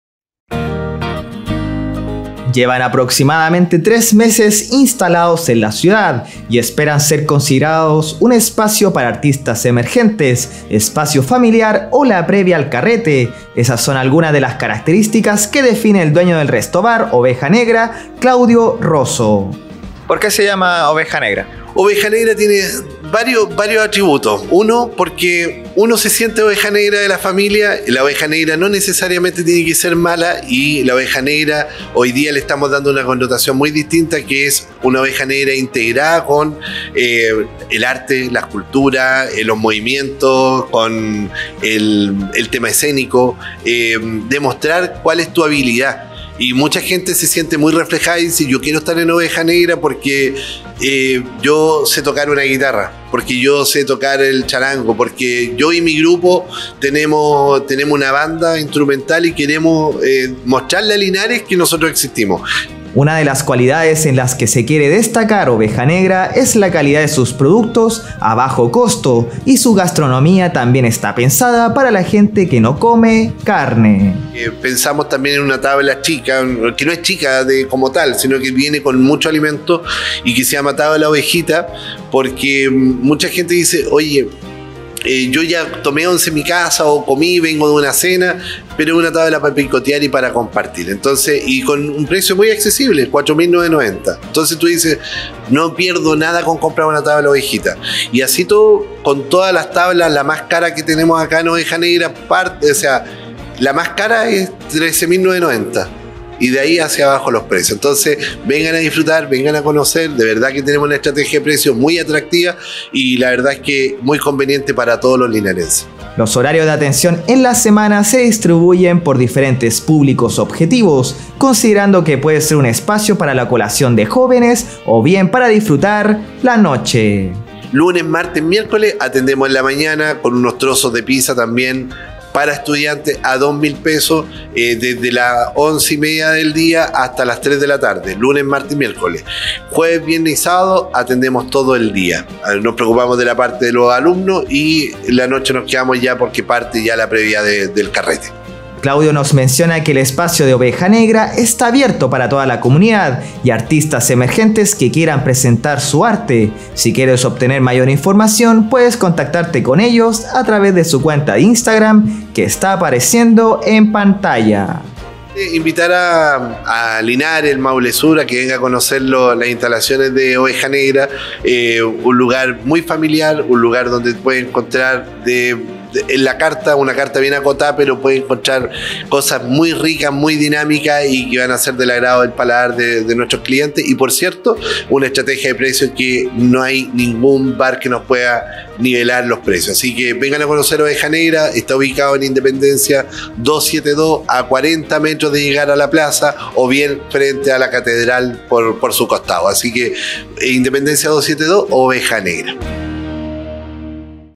Restobar Oveja Negra busca ser un espacio para artistas emergentes | Martes 25.04.23
Llevan aproximadamente 3 meses instalados en la ciudad y esperan ser considerados un espacio para artistas emergentes, espacio familiar o la previa al carrete; esas son algunas de las características que define el dueño del restobar Oveja Negra, Claudio Rosso.

Nota de prensa emitida el día martes 25 de abril del 2023 a través de nuestro noticiero central Maule Informa.